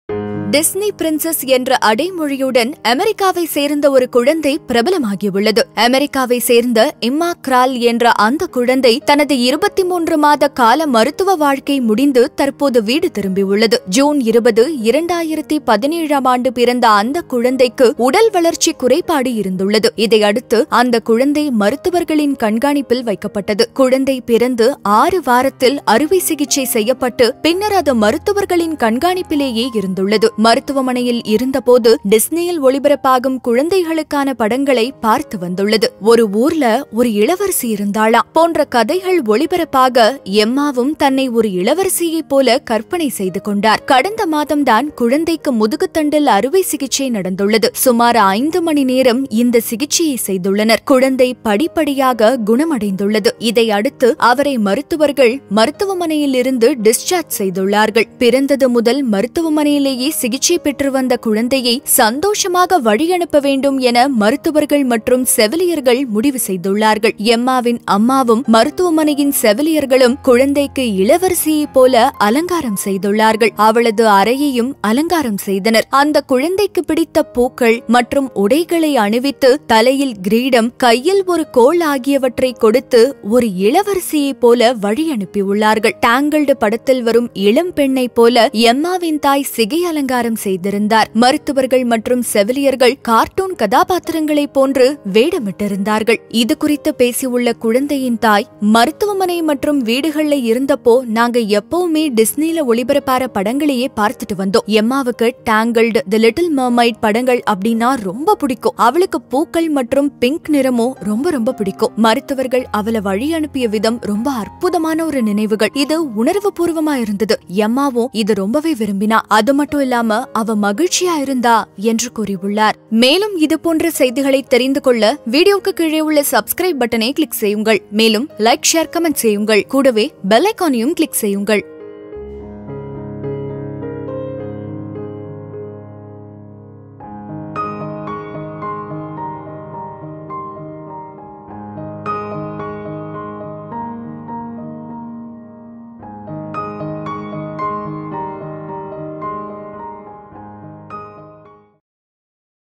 `ஒரு ஊர்ல ஒரு இளவரசி இருந்தாளாம்..!’ மருத்துவமனையில் ஓர் நெகிழ்ச்சி சம்பவம்
‘டிஸ்னி பிரின்சஸ்’ என்ற அடைமொழியுடன் அமெரிக்காவைச் சேர்ந்த ஒரு குழந்தை பிரபலமாகியுள்ளது. அமெரிக்காவைச் சேர்ந்த Emma Krall என்ற அந்தக் குழந்தை, தனது  23 மாத கால மருத்துவ வாழ்க்கை முடித்து தற்போது வீடு திரும்பியுள்ளது.